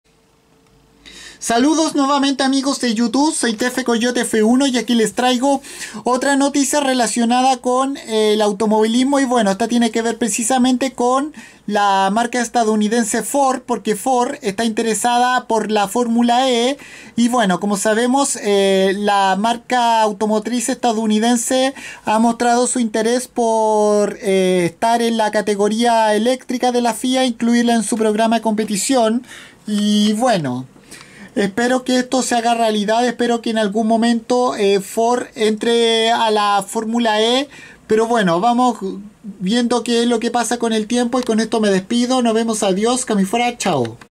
Ford está interesada en entrar a la Formula E
La marca estadounidense quiere ingresar a la categoría eléctrica de la Fia en algún momento